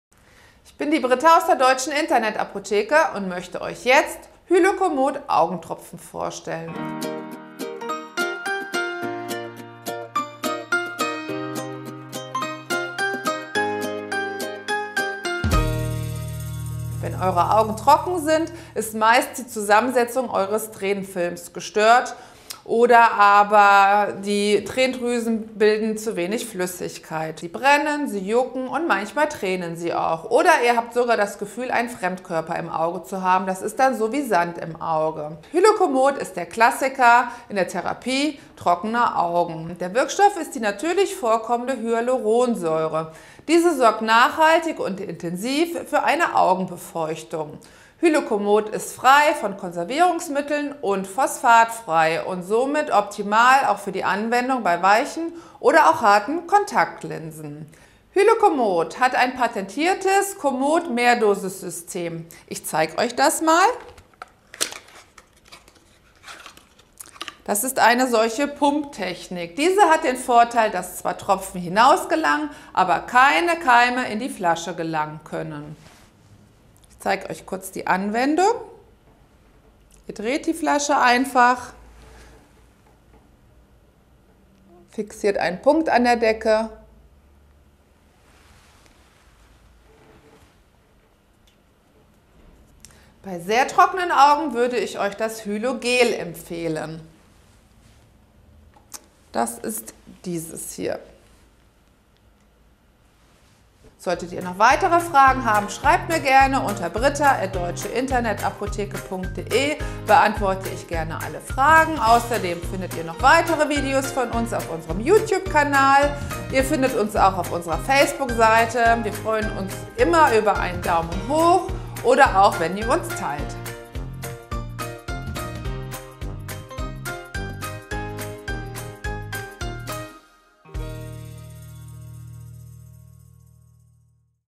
Hylo Comod - Anwendung und Dosierung - Deutsche Internet Apotheke
Hylo Comod - zur Anwendung am trockenen Auge - in diesem Video kurz &  knackig beschrieben.

Gut informiert durch Eure Deutsche Internet Apotheke - die Versand Shop Apotheke.

Das Video dient nicht zu Werbezwecken.